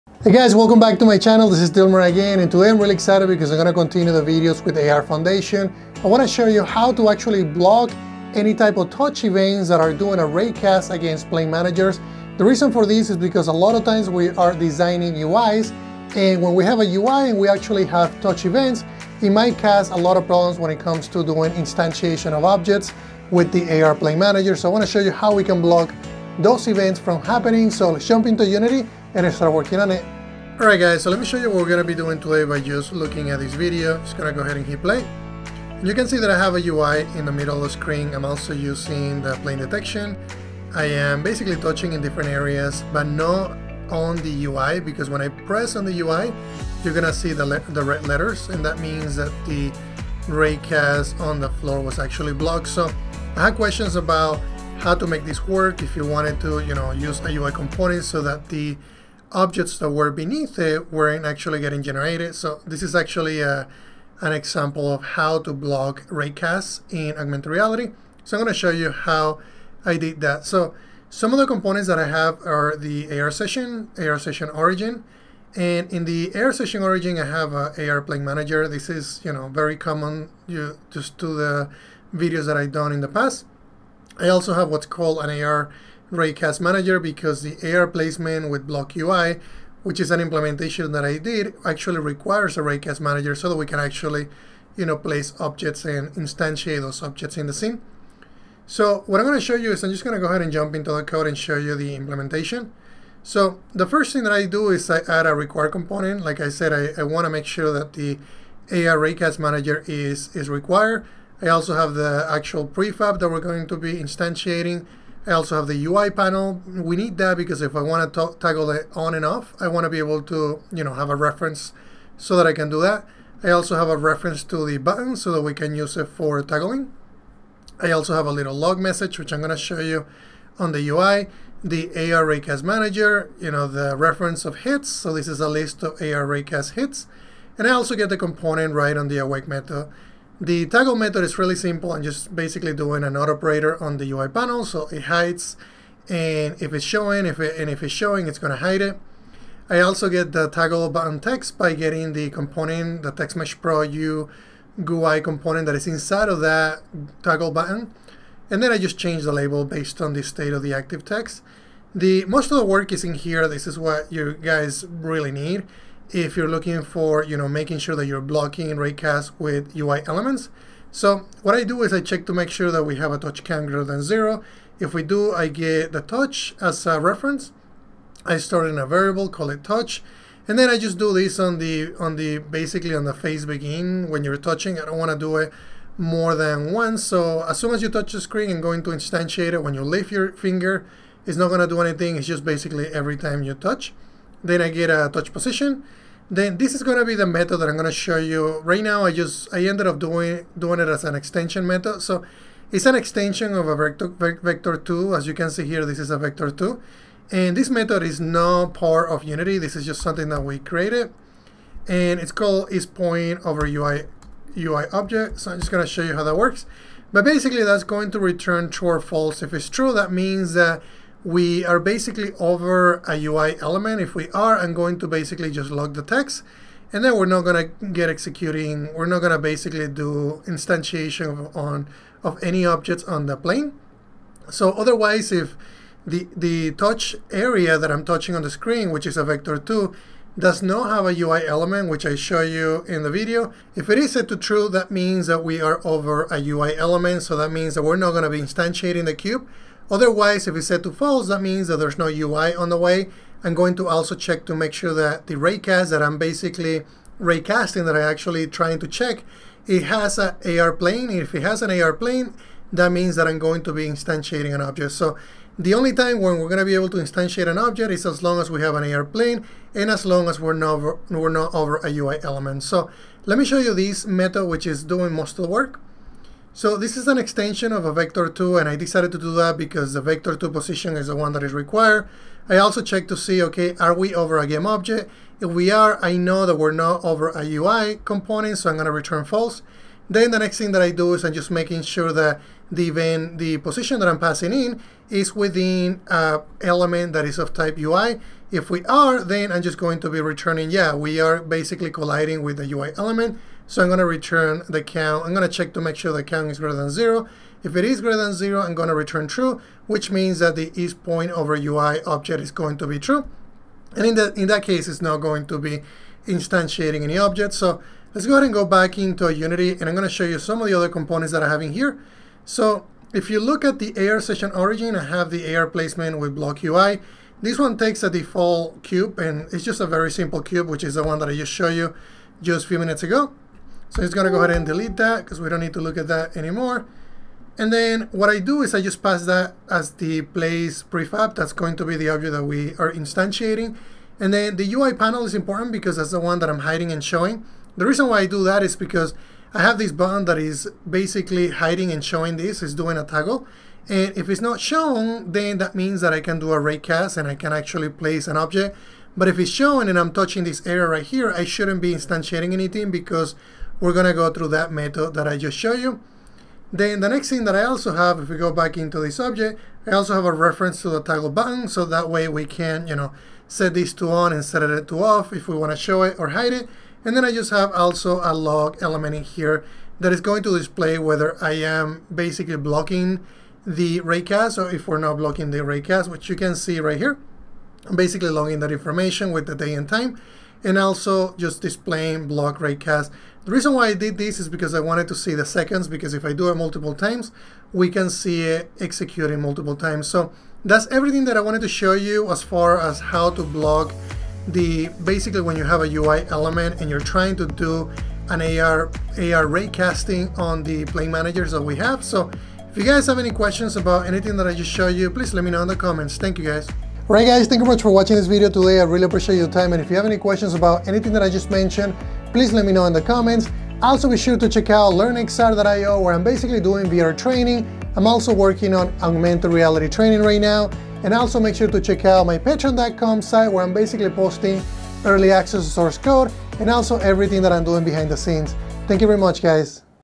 Unity3d AR Foundation - How To Block AR Raycast From UI Touch Events? (For ARCore And ARKIT)
Today I provide you with a demo on how to block touch events from instantiating AR objects when touch events collide with UI components. This video also shows you how to create a C# extension from a Vector2 which extends its functionality and adds a new method to check for UI collisions.